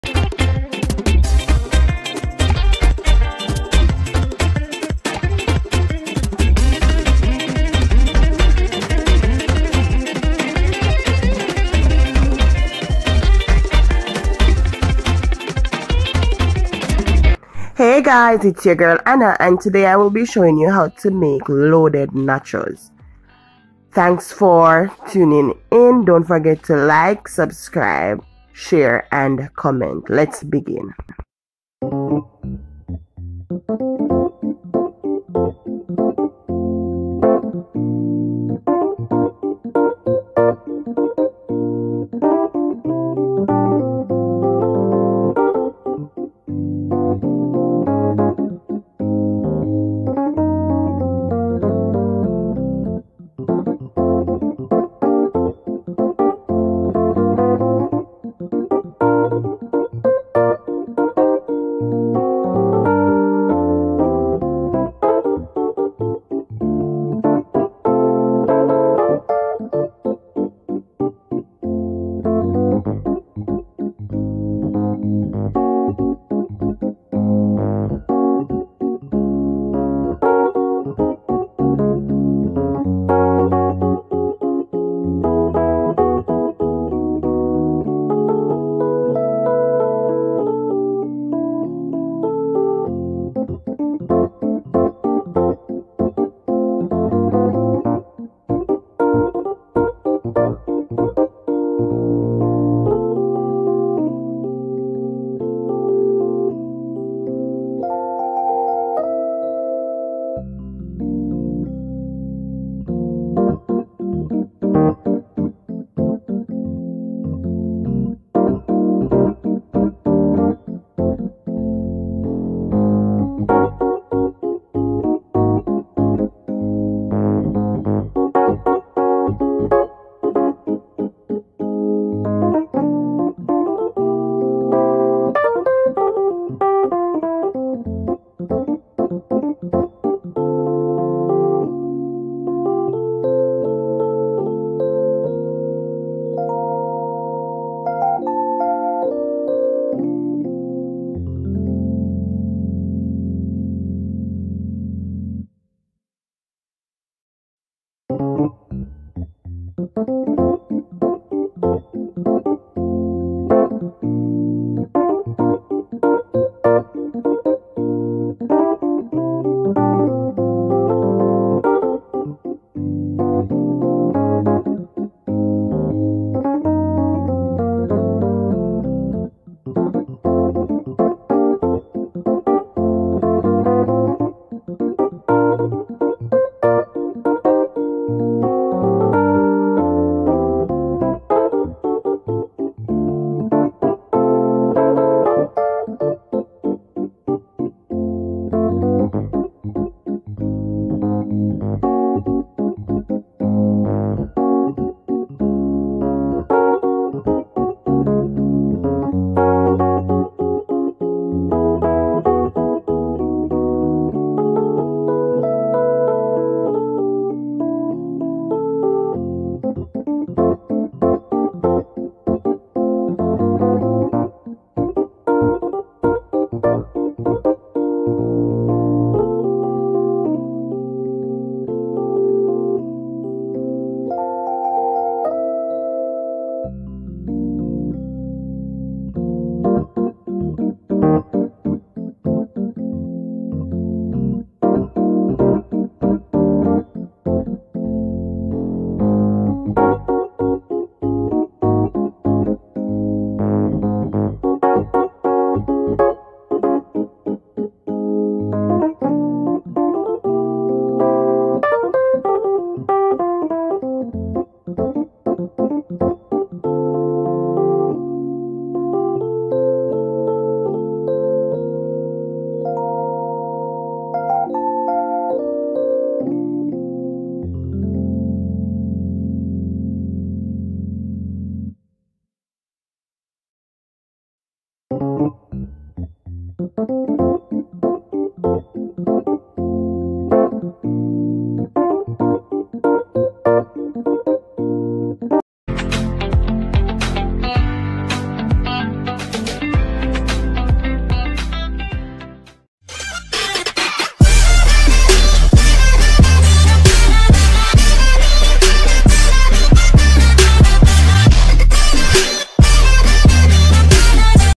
Loaded Nacho///WITH FRY PLANTAINS AND NUGGETS
This video consist of
Cabbage
Carrot
Tomato
Cheese
Nuggets
Mayo
Yougut and mint
Ripe Plantain
Onion
Vinegar